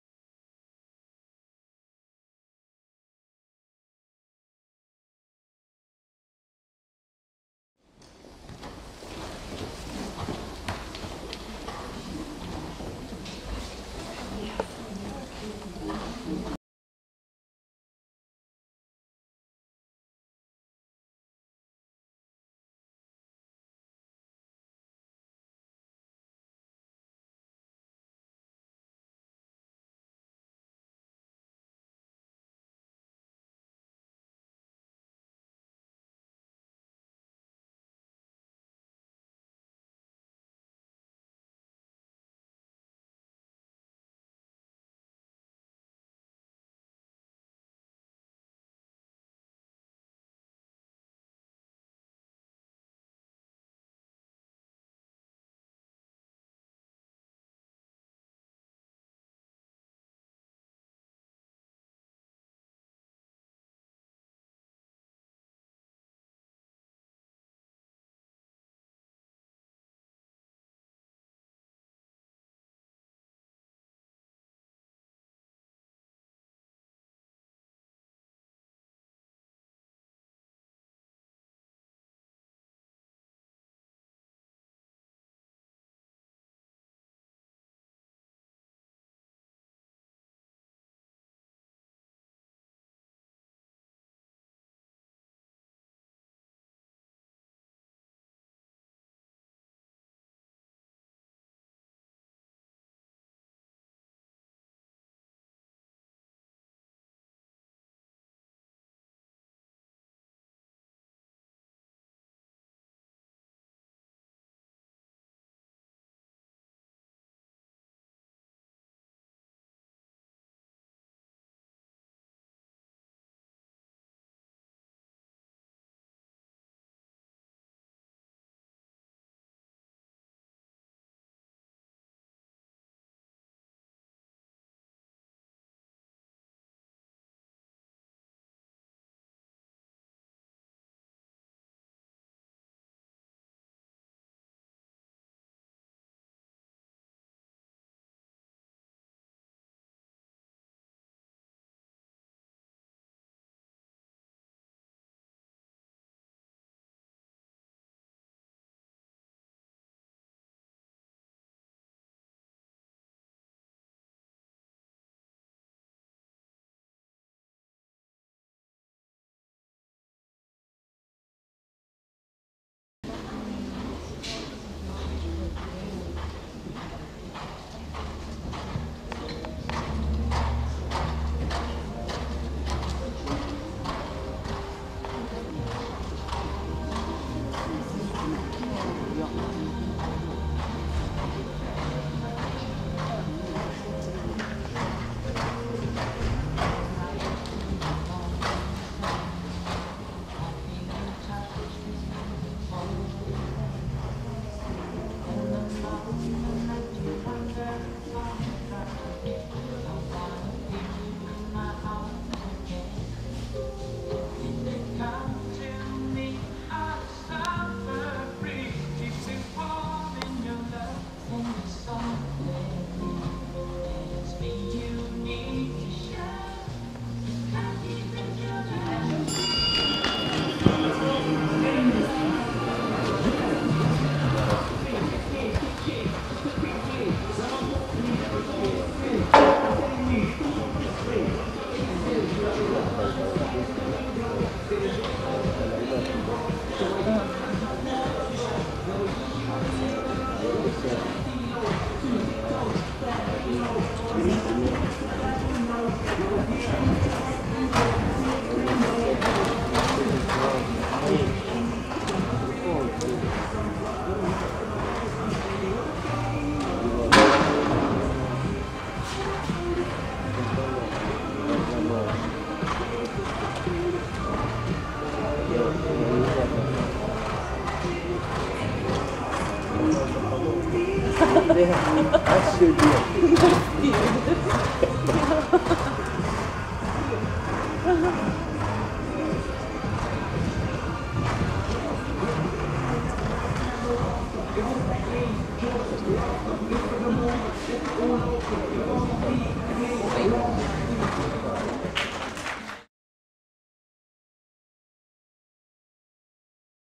Dvikove Belmonte 2010-03-28, Greta Sapkaite-Zvaigzdynas
Nacionalinės jojimo trikovės federacijos 2010 m. Lietuvos žiemos dvikovės čempionatas, Belmonto dvikovės taurė Belmonte, Vilnius, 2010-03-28. Joja Greta Šapkaitė su Žvaigždynu.
Advokato Raimondo Pakšio interneto svetainė yra , o YouTube kanalas - YouRaimis ( @raimondas.paksys ) .